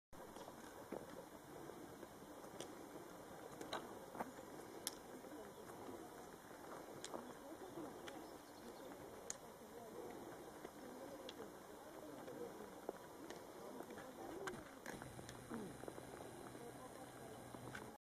Camino de Santiago - Hacia Triacastela
180 kilómetros de Camino de Santiago. 2 al 10 de abril de 2010.